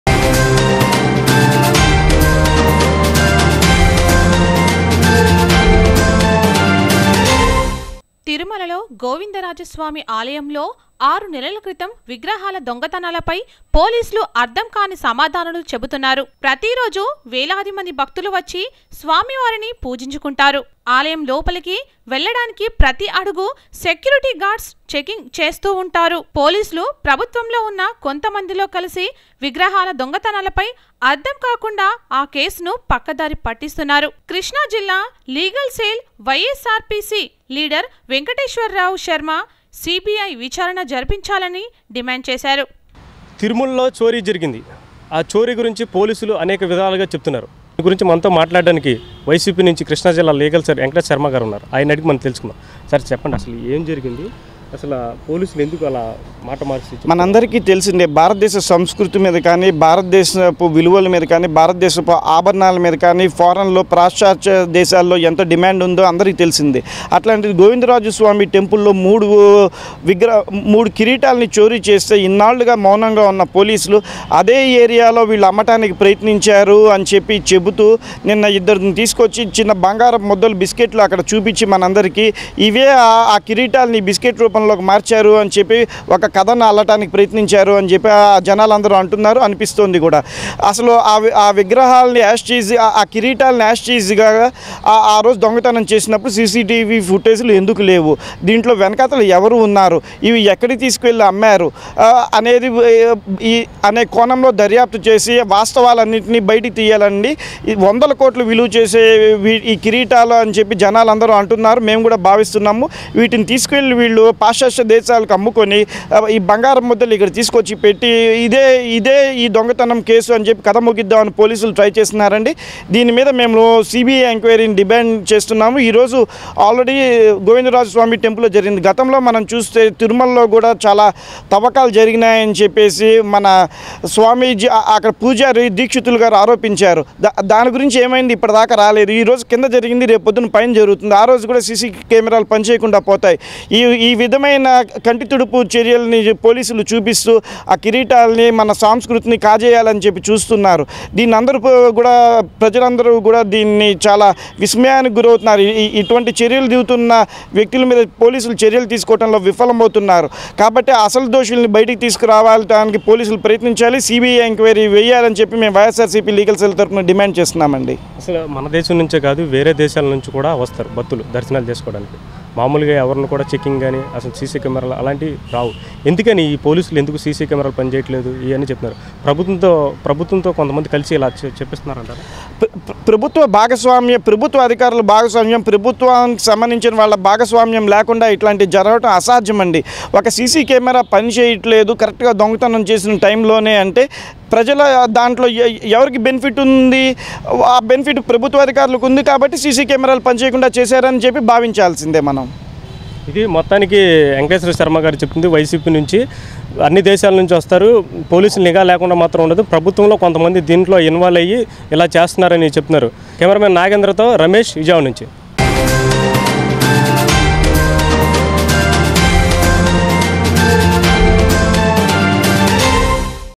GOVINDARAJA SWAMY ALAYA CHORY CASE CBI KI APPAGINCHALANI DEMAND
తిరుమలలో గోవిందరాజస్వామి ఆలయంలో లో ఆరు నెల క్రితం విగ్రహాల దొంగతనాలపై పోలీసులు అర్థం కాని సమాధానాలు చెబుతున్నారు .. ప్రతిరోజు వేలాది మంది భక్తులు వచ్చి స్వామివారిని పూజించుకుంటారు ఆలయం లోపలికి వెళ్లడానికి ప్రతి అడుగు సెక్యూరిటీ గార్డ్స్ చెకింగ్ చేస్తూ ఉంటారు.పోలీసులు ప్రభుత్వంలో  ఉన్న కొంతమందితో కలిసి విగ్రహాల దొంగతనాలపై అర్థం కాకుండా ఆ కేసును పక్కదారి పట్టిస్తున్నారు.
కృష్ణాజిల్లా లీగల్ సెల్ వైఎస్సార్సీపీ లీడర్ వెంకటేశ్వరరావు శర్మ సి.బి.ఐ విచారణ జరిపించాలని డిమాండ్ చేశారు.